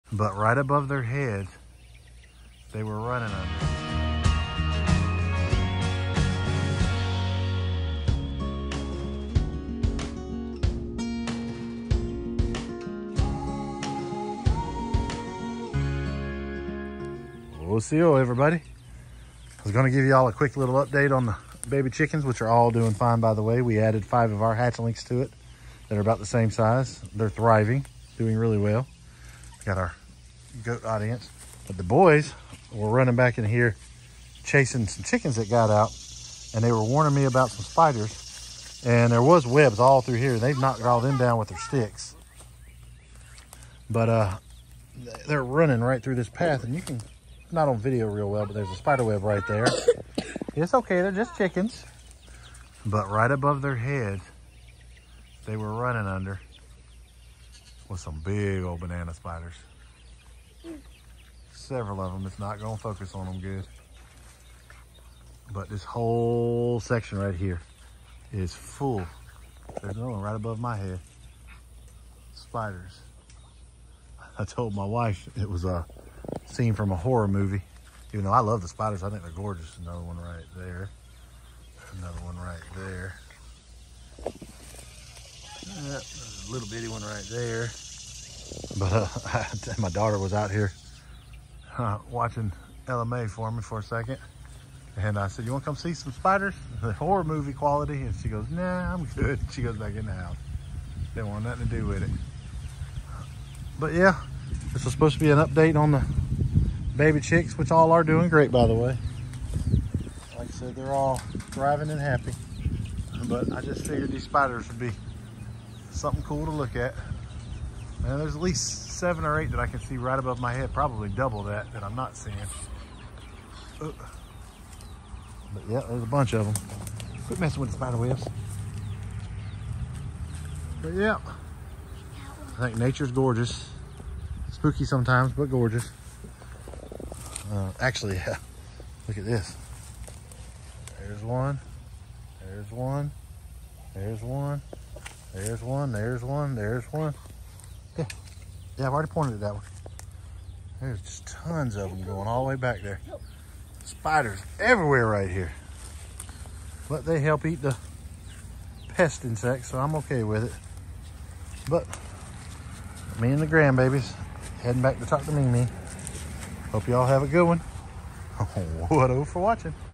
Spiders!!!
Boys were chasing some wandering chickens and found a spider web and asked me to see it and there is actually dozens of thse golden orb weavers. Also the new chicks are doing great. Wado for Watching